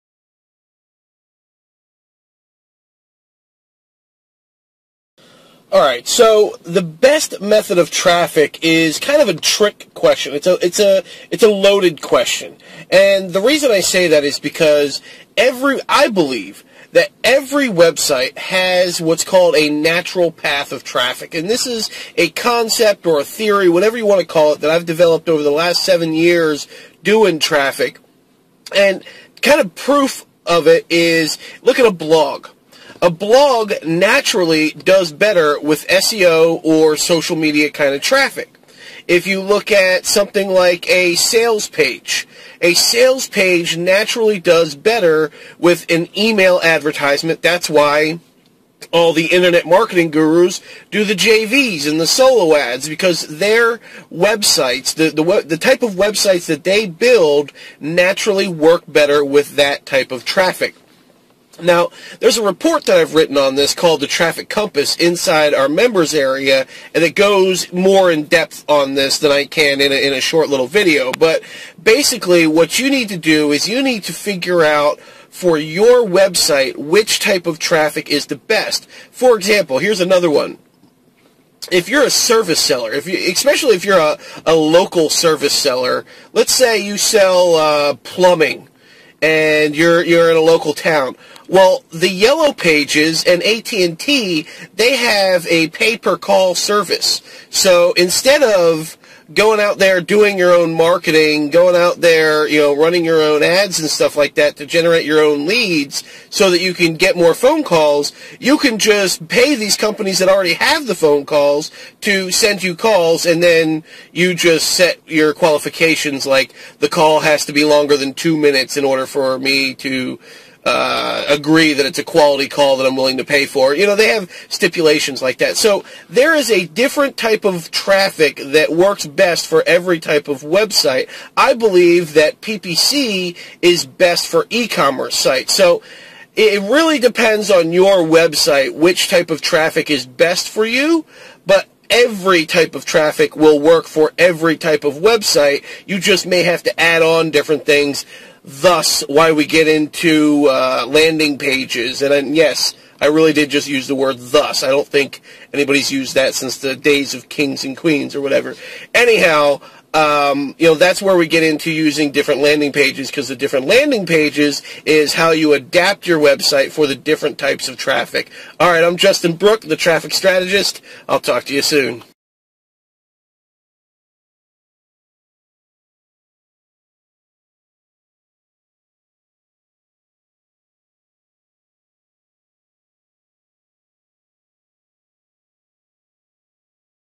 If I Could Choose Only One Traffic Source
This video discusses which traffic source is the best traffic source and how that's a loaded question. Plus how to make all traffic sources work for your website.